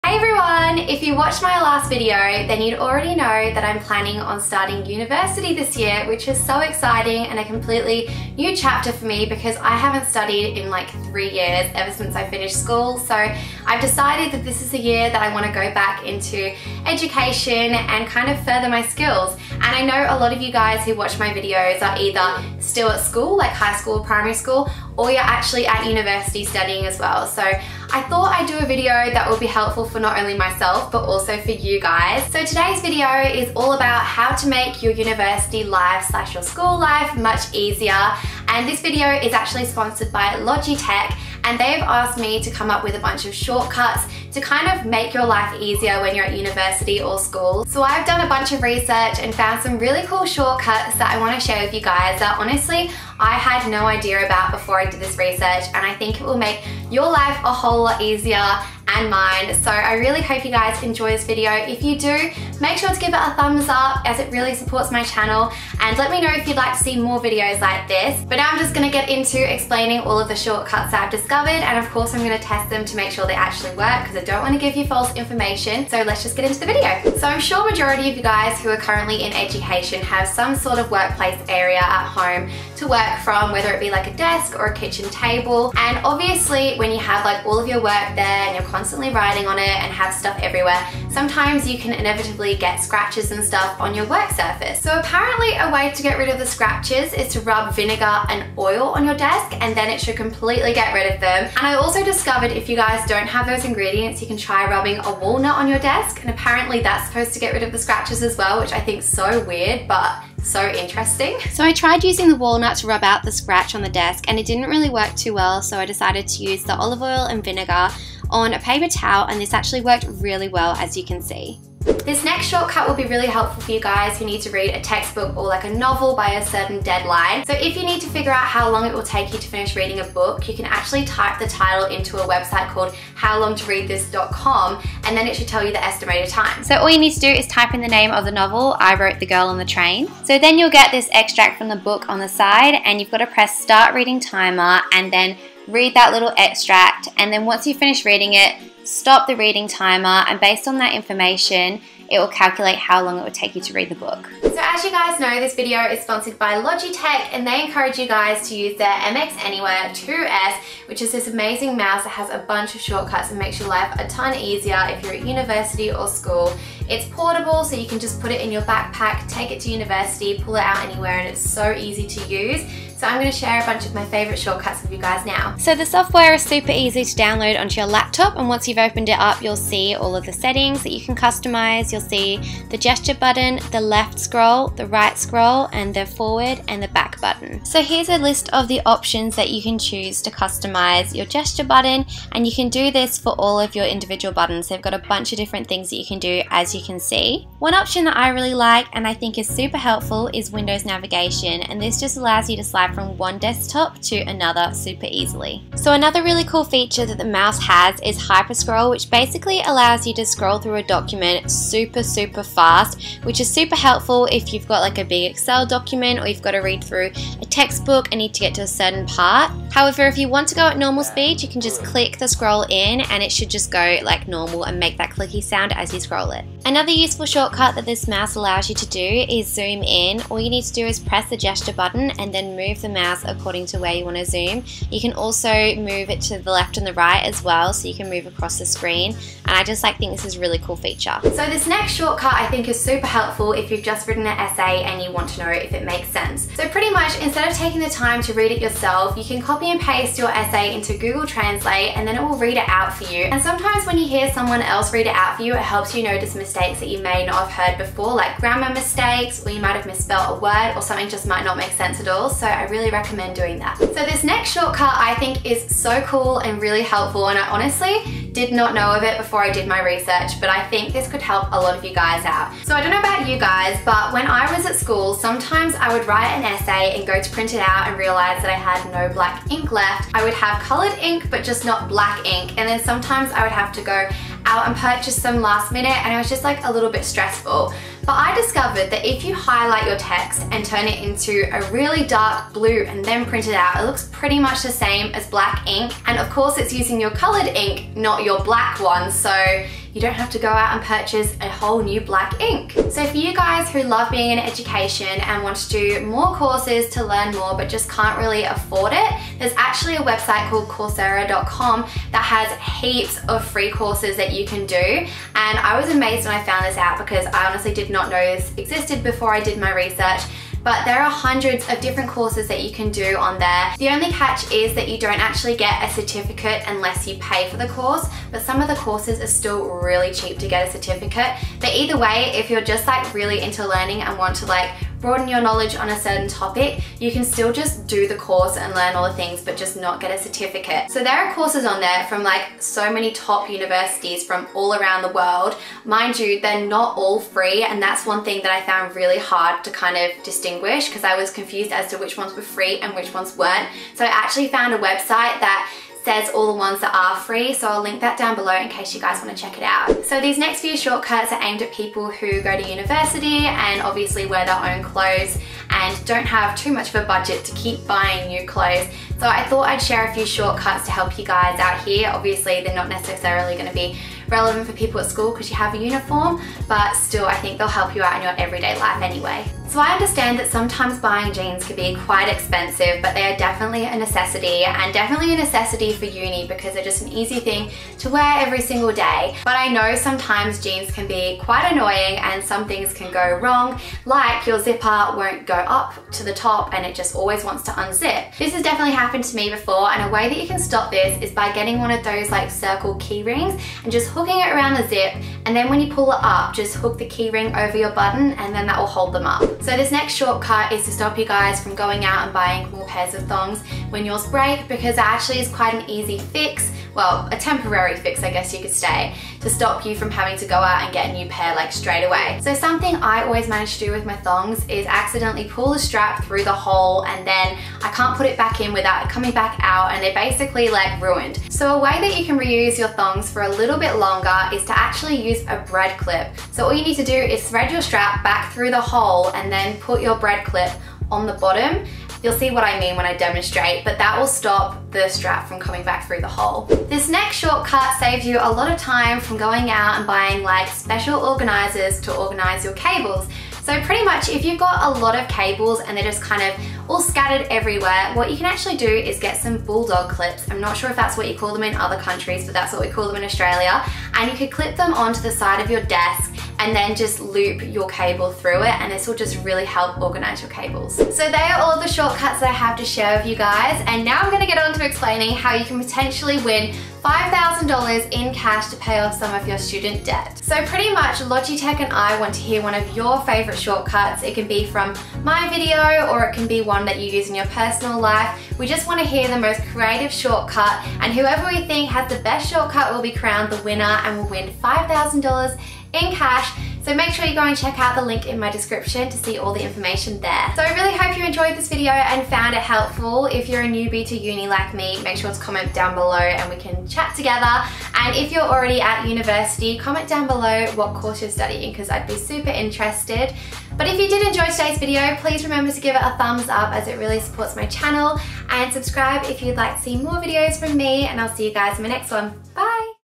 Make Uni Life Easier With These Shortcuts + Win $5000!
Logitech now want to hear what your favourite shortcut is! Tell them your own personal favourite shortcut for your chance to win $5,000 off your student debt, and take home the coveted Shortcut Crown.

ella_victoriaa

MAILING ADDRESS:

Sutherland
NSW
Australia
1499